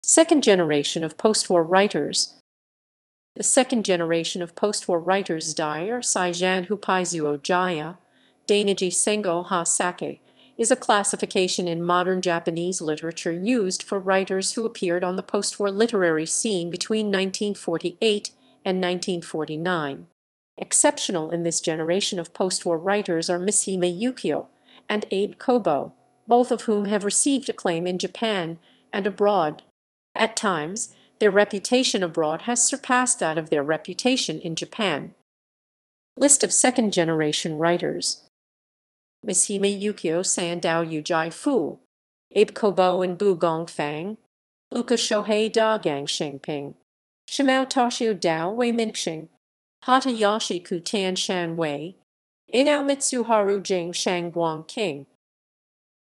Second Generation of Postwar Writers
The Second Generation of Postwar Writers (第二次戦後派作家, Dainiji Sengo-ha Sakka) is a classification in modern Japanese literature used for writers who appeared on the postwar literary scene between 1948 and 1949.
Exceptional in this generation of postwar writers are Mishima Yukio and Abe Kōbō, both of whom have received acclaim in Japan and abroad. At times, their reputation abroad has surpassed that of their reputation in Japan.